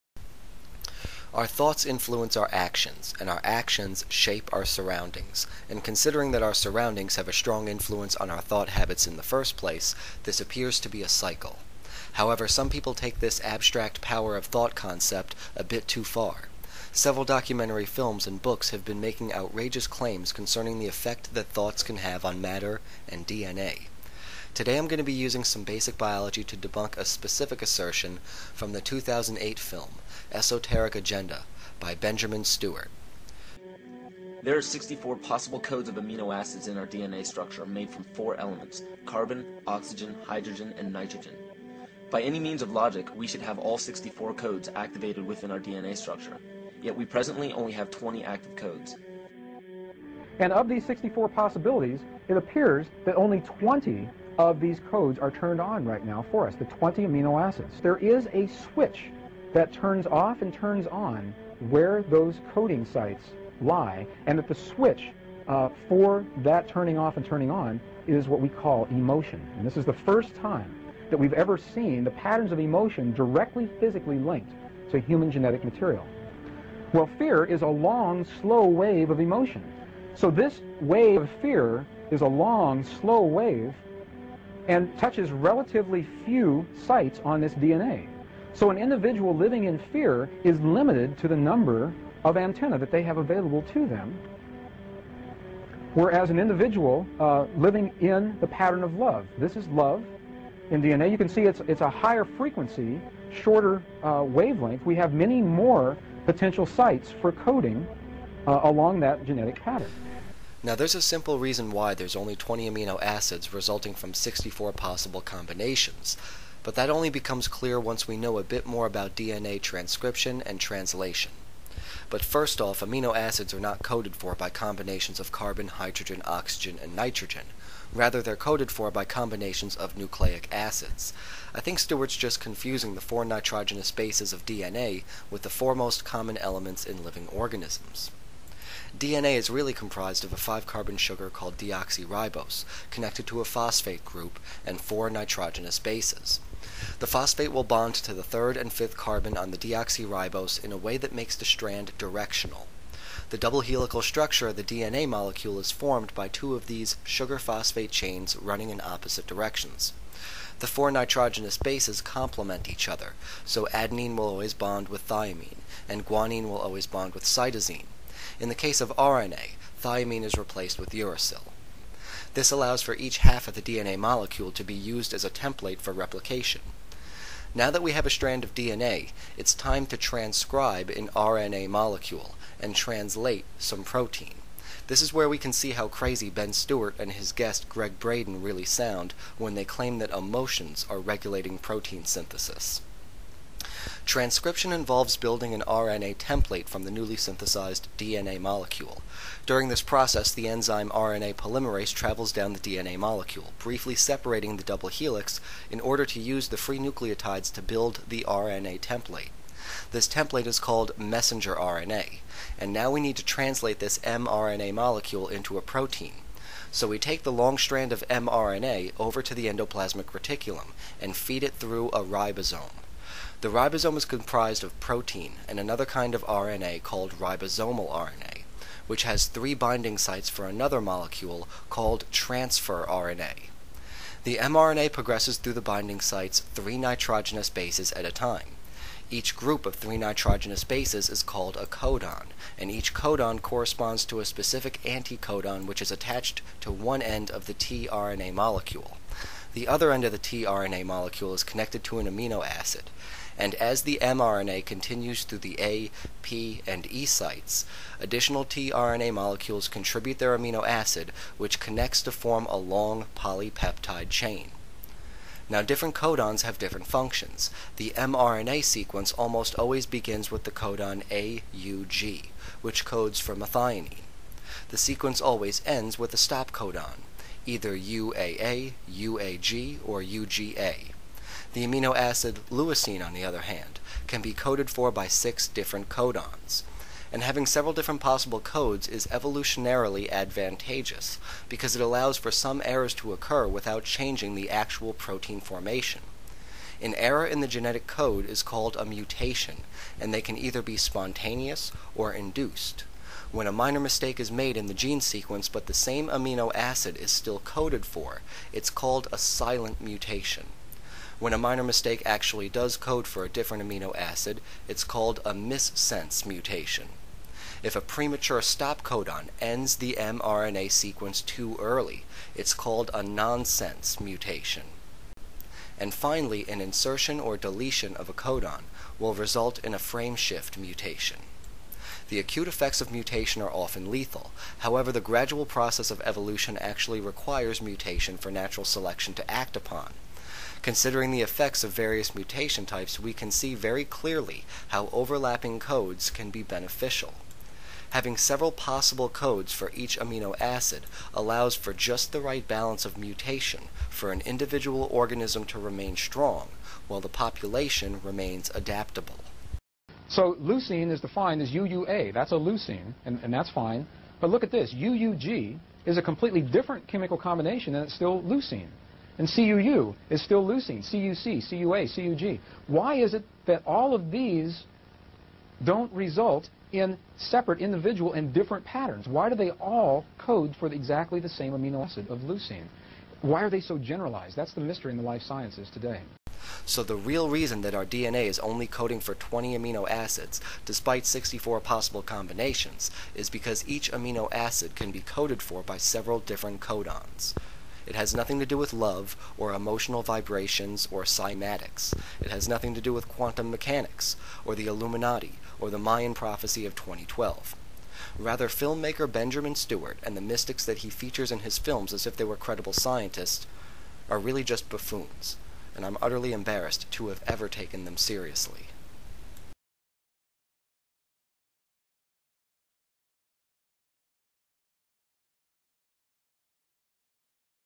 Do emotions control DNA? Debunking Gregg Braden
In the 2008 internet film Esoteric Agenda, it is suggested that emotions and beliefs control DNA. If this premise were true it could provide a foundation for a variety of other conspiracy theories and new age nonsense.

1-It gives the NWO the necessary motivation to implement psychological warfare.
2-It explains why pseudo medicine like acupuncture and homeopathy doesn't work on skeptics.
3-It gives Big Pharma and the FDA a reason to suppress all the supposed research in support of faith healing and alternative medicine.
4-And it supports this New Age quantum consciousness garbage that you see in media like The Secret, What the Bleep, Biology of Belief, and many others.

Despite the popular New Age belief, our emotions do not control our genes nor do they influence matter at the quantum level. Here's why.

To learn more about Biology and many other interesting topics, I recommend the Crash Course Channel. Below is a link to their episode on DNA transcription and translation.